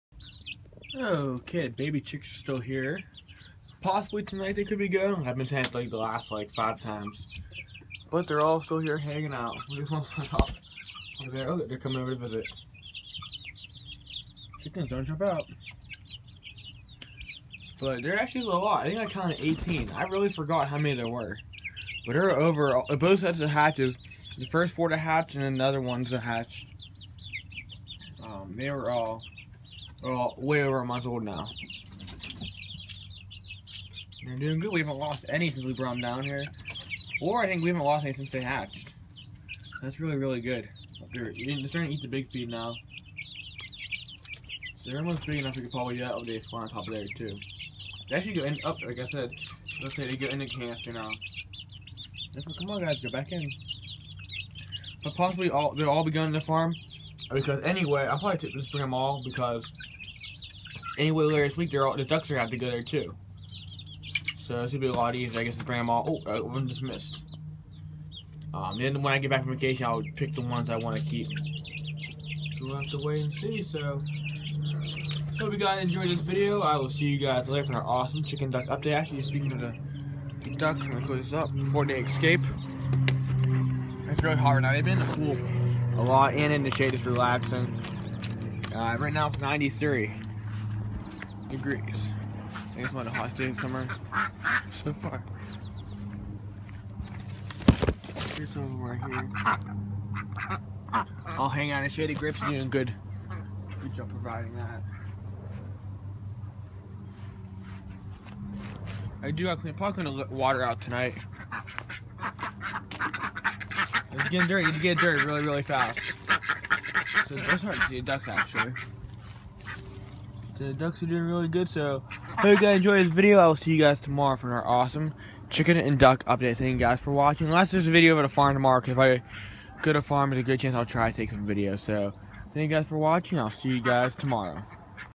Hatching and Raising DUCKS 2015!! Daily VIDEOS (CHICKS LEAVE TOMORROW!!!) Day 78
Daily videos of my ducks living the duck dream! In the next few weeks I’m getting hatching eggs from the farm again, so by the end of June I may have over 70+ baby chickens and ducks!! For the first time ever I’ll be hatching eggs from my own ducks (Or Pets) I’ve been wanted to do this for a very long time so I’m very excited to see my pets have babies of their own. Hope you guys are ready for an awesome summer with me watching these guys grow!!!